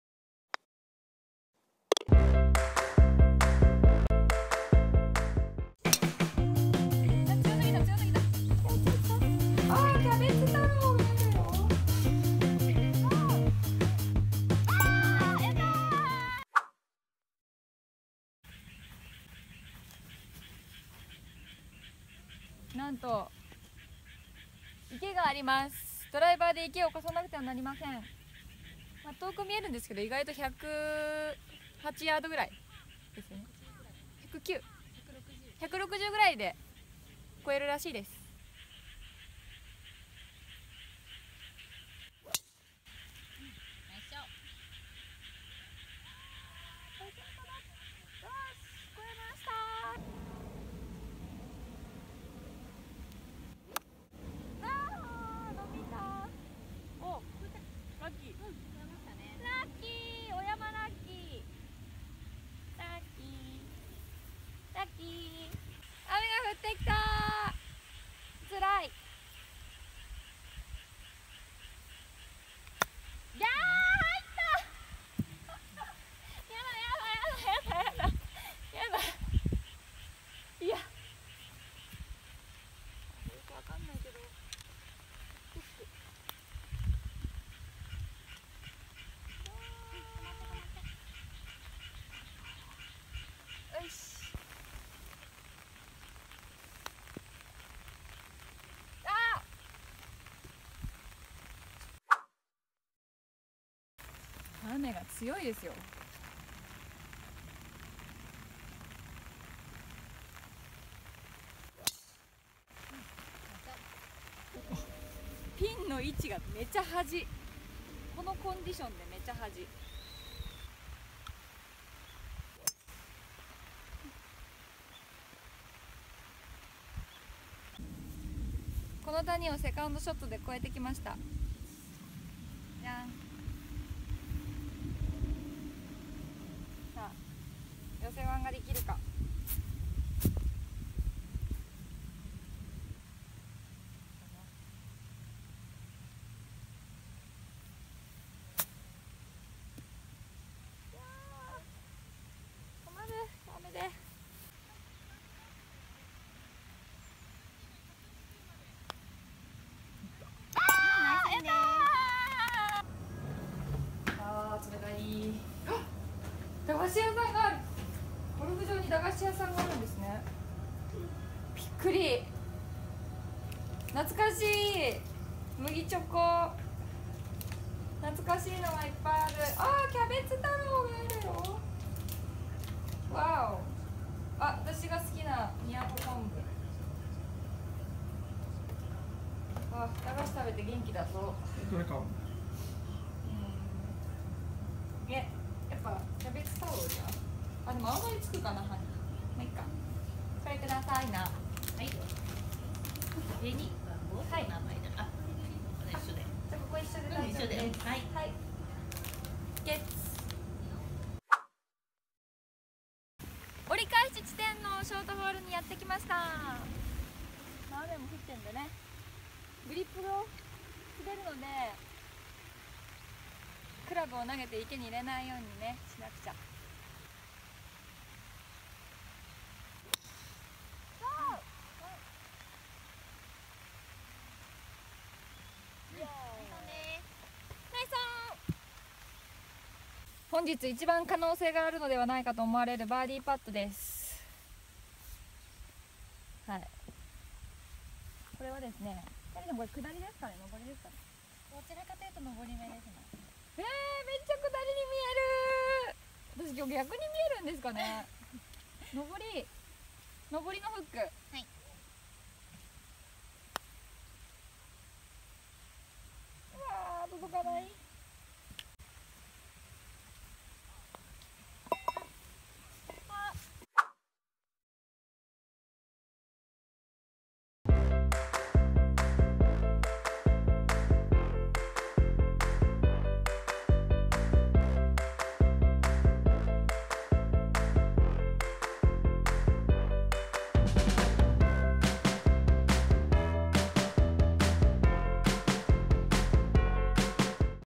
5番の大きな谷、からのキャベツ太郎【きみさらずゴルフリンクス】[4-6H] 三枝こころ
ゴルフ女子をどんどん増やしたい♡

♣今日の動画
難コースで評判のきみさらずゴルフリンクス、レギュラーティーでラウンドしています。

4H 311yd PAR4
5H 354yd PAR4
6H 393yd PAR4

もうお気づきかと思いますが、このコースはフェアウェイもラフもうねうねとうねっていて、アンジュレーション(斜面)の強いゴルフ場です。グリーンの近くにつけたとしても寄せるのが難しい。

注目は5番ホール、二打目からは大きな谷を超えなくてはいけません。
ウッドでうまく超えられるといいけれど、、、

途中の売店には駄菓子コーナーがあり、懐かしのキャベツ太郎に出会いました。

♣本日のゴルフ場: きみさらずゴルフリンクス

♣日々のゴルフのことを書いています。